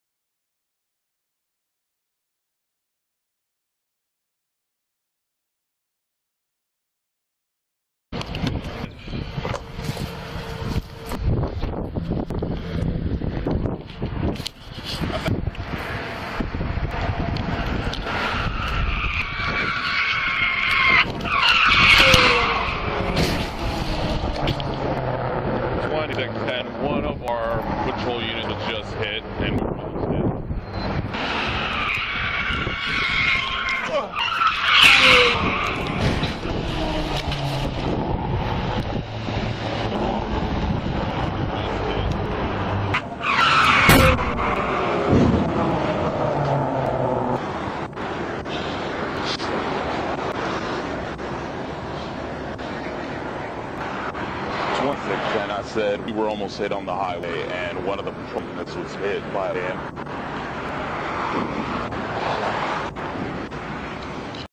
Police squad car hit while officers were safeguarding drivers
The Carrollton Police Department wants to remind drivers to slow down and move over.
 The department posted dash cam video on Facebook showing two of their officers attempting to safeguard drivers by removing a roadway obstruction on I-35E last month.
 In the process, Officer Tillman and Officer Villegas are seen jumping over a barrier on the shoulder of I-35 to move out of the way from being hit by a car heading their direction.
 The driver was not hurt and neither were the officers.
 According to police, the driver was not drunk but was issued multiple citations.

BuckleupDaily & Drive Safely!
Latest Car Accidents and Crashes
Bucklupdaily.org
!قد بأمان!